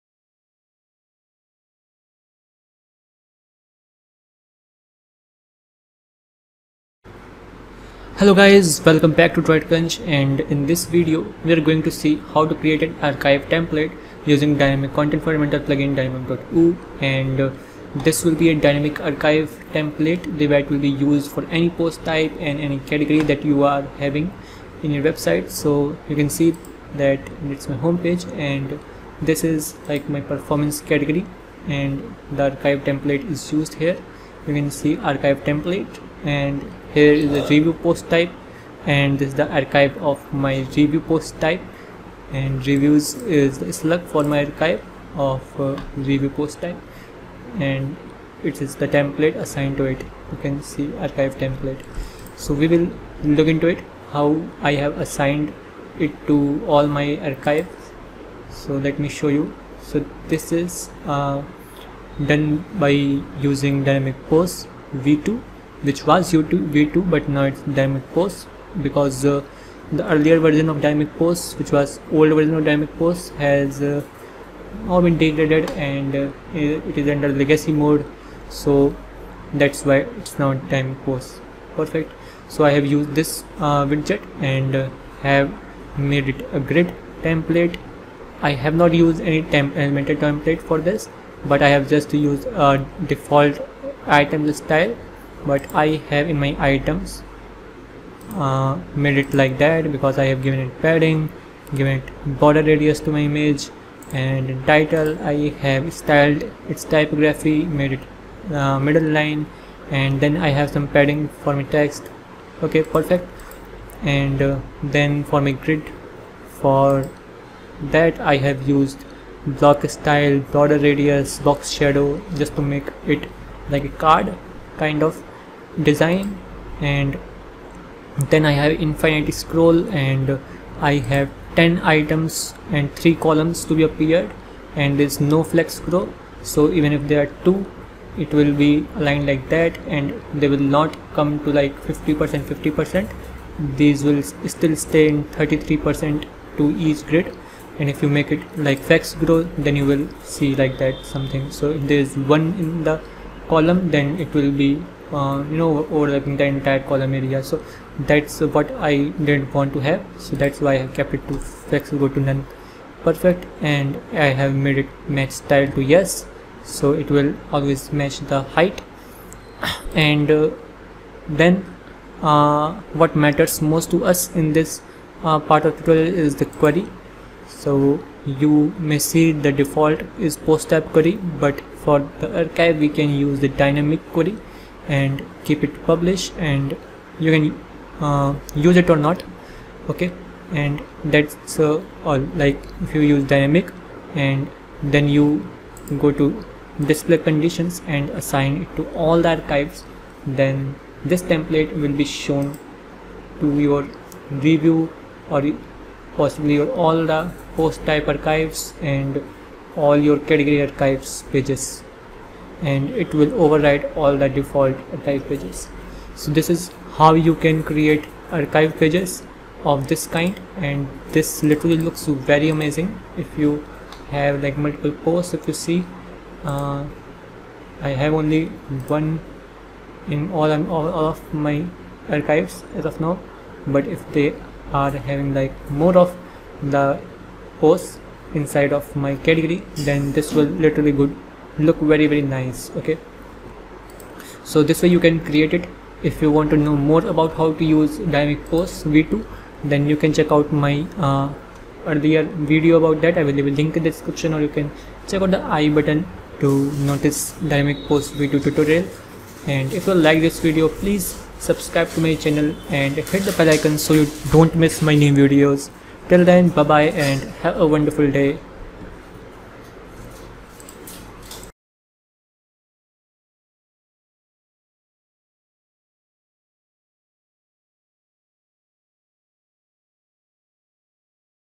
How to Create Archive Pages with Dynamic Posts (Dynamic.ooo) and Elementor Builder
Learn how to create impressive archive pages with dynamic posts v2 widget by Dynamic.ooo and Elementor Page builder. This video also explains how to customize your archive page to the fullest to get the best out of it.